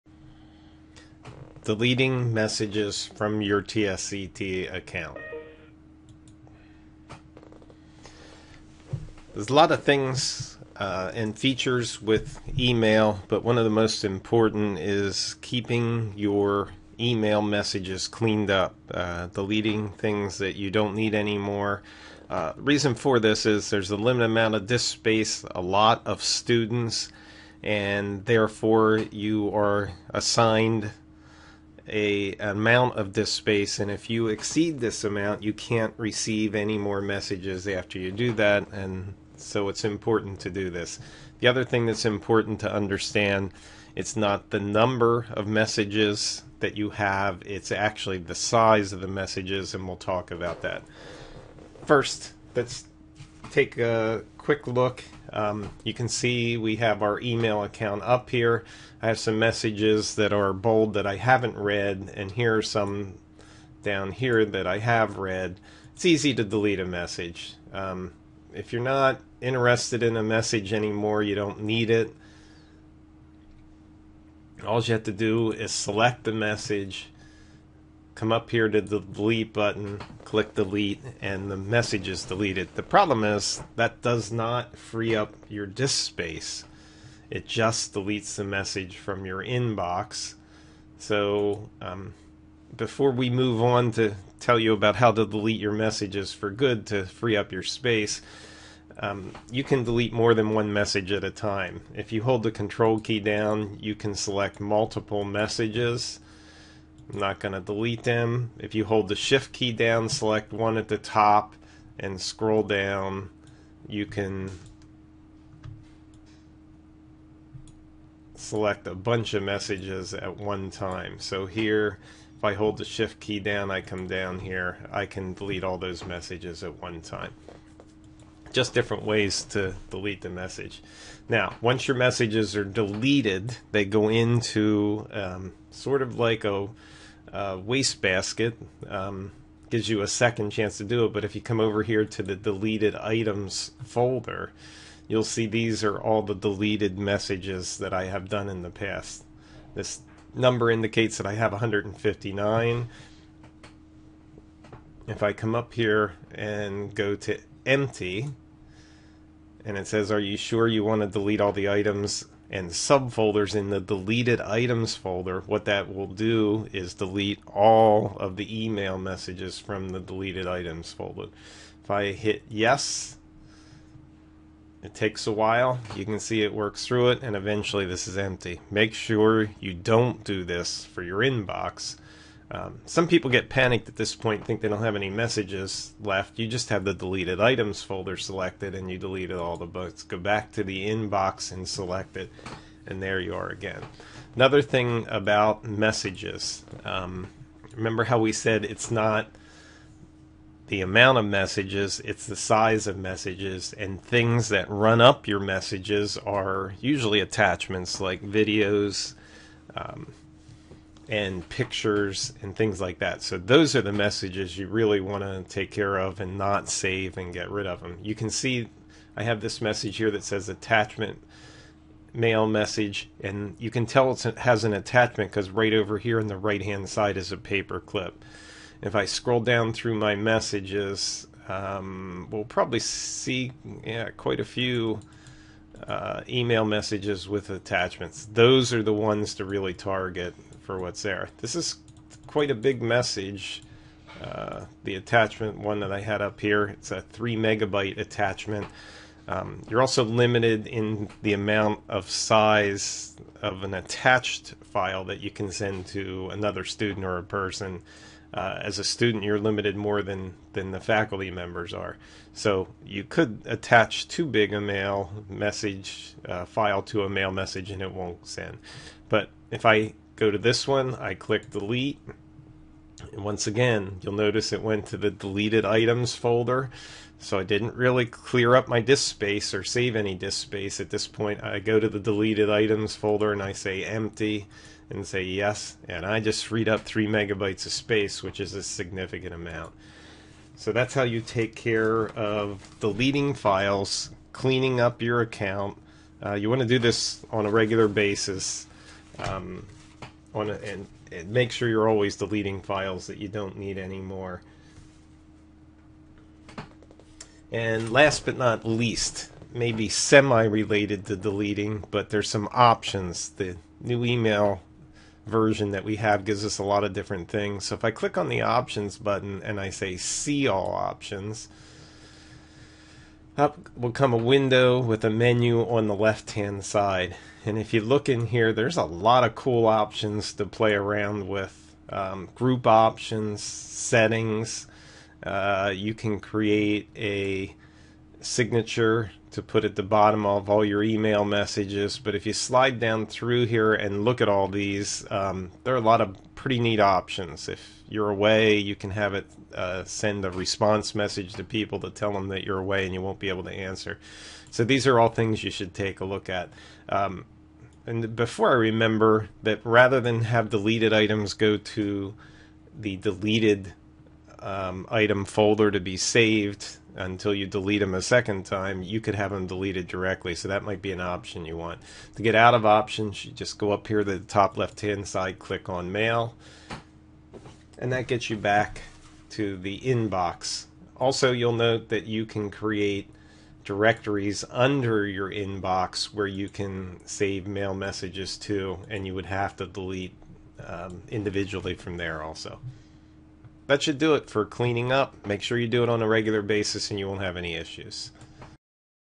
Deleting Messages from your TSCT E-mail
How to delete messages from your TSCT e-mail account so that you do not exceed the maximum file size limitations for your mailbox